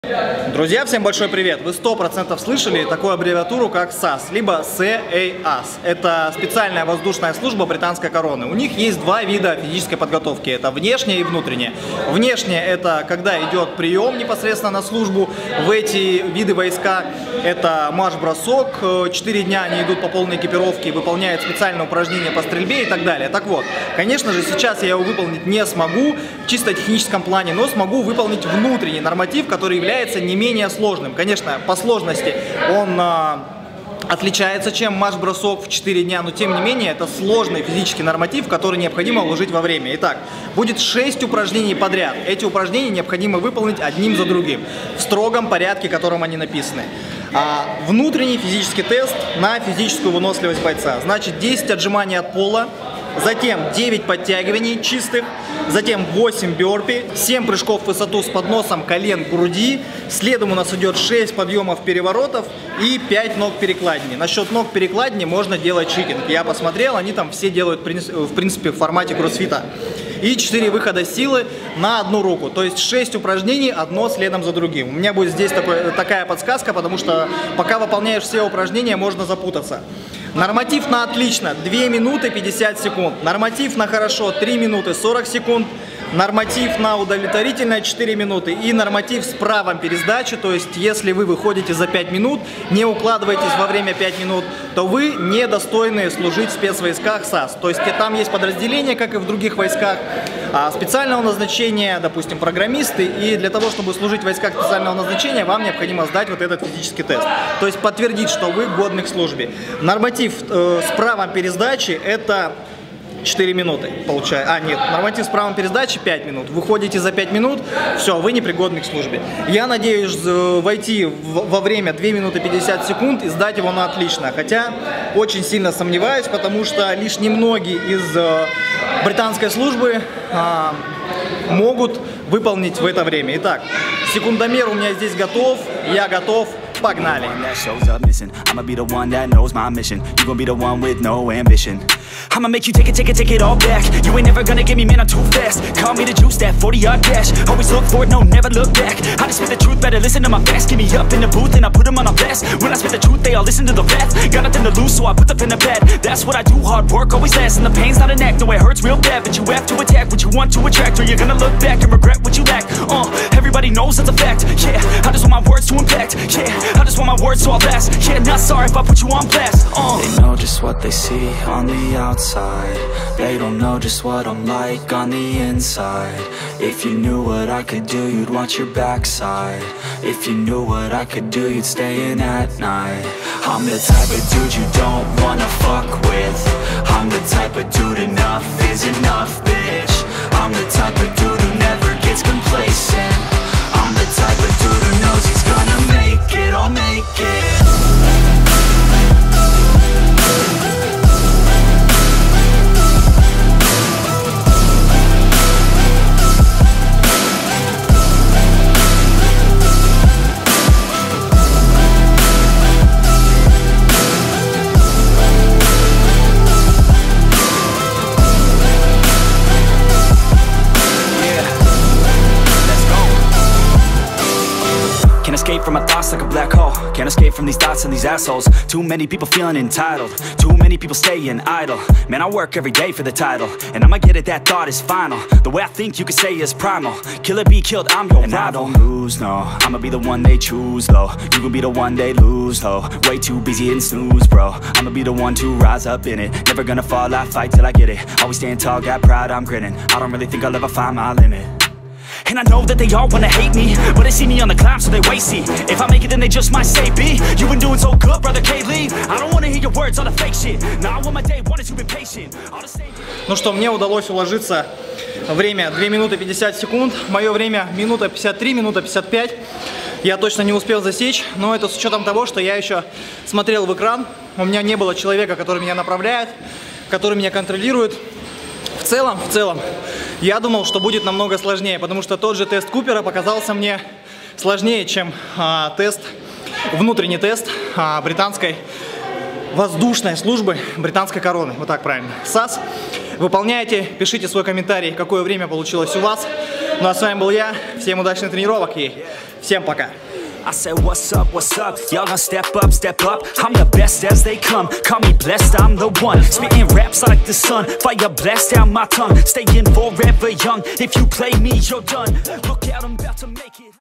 СОЛДАТ ВДВ РФ ВЫПОЛНЯЕТ ТЕСТ ФИЗ ПОДГОТОВКИ SAS / БРИТАНСКИЕ СПЕЦСЛУЖБЫ
Десантник выполняет норматив британских спецслужб.

Норматив Специальной воздушной службы британской короны:
- 10 ОТЖИМАНИЙ ОТ ПОЛА
- 9 ПОДТЯГИВАНИЙ
- 8 БЁРПИ
- 7 ПРЫЖКОВ С ПОДНОСОМ КОЛЕН К ГРУДИ
- 6 ПОДЪЁМ С ПЕРЕВОРОТОМ
- 5 ПОДНОСОВ НОГ К ПЕРЕКЛАДИНЕ
- 4 ВЫХОДА НА ОДНУ

НОРМАТИВ НА ОТЛИЧНО - 2,50
НА ХОРОШО - 3,40
НА УДАВЛЕТВОРИТЕЛЬНО - 4 МИН